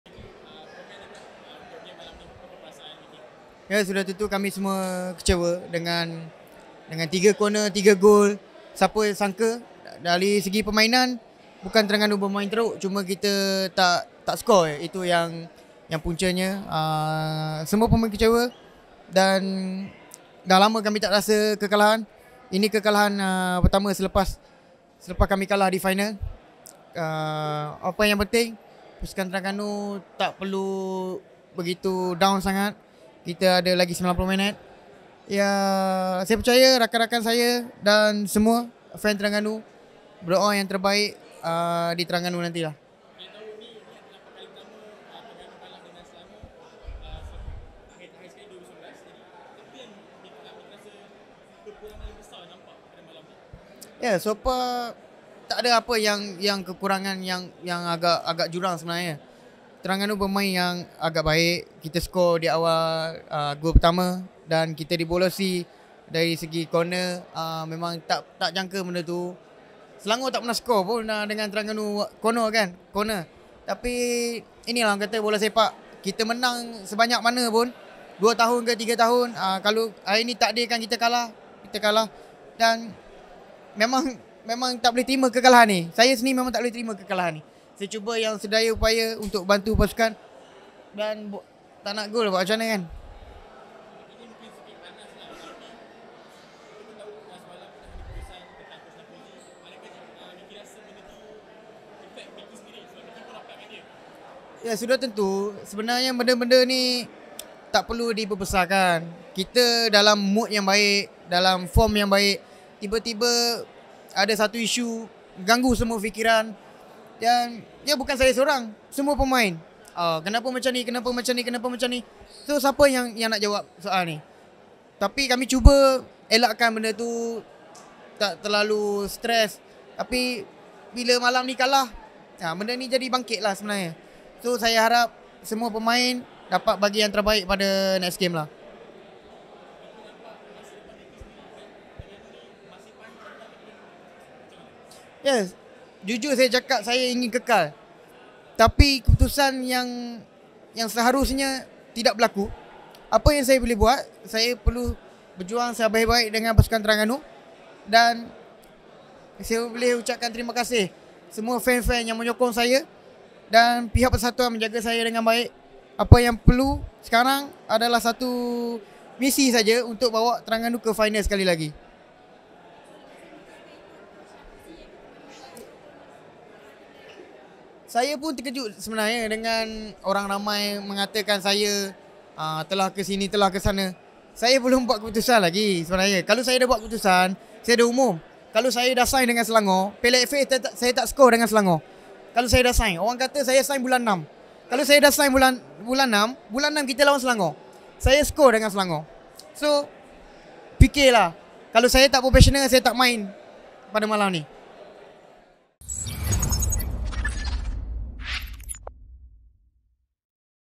"Kalau saya tak professional, saya tak main malam ini" -Faisal Halim
Temu bual eksklusif bersama pemain sayap buat pasukan Terengganu FC, Faisal Halim selepas perlawanan menentang Selangor FC.

Faisal Halim turut menjawab dakwaan orang ramai apabila dikaitkan dengan perpindahan ke pasukan Selangor bagi musim depan.

Sebagi rekod, Terengganu FC tewas di tangan Selangor FC yang mengekalkan rekod tanpa kalahnya di Stadium MBPJ malam ini.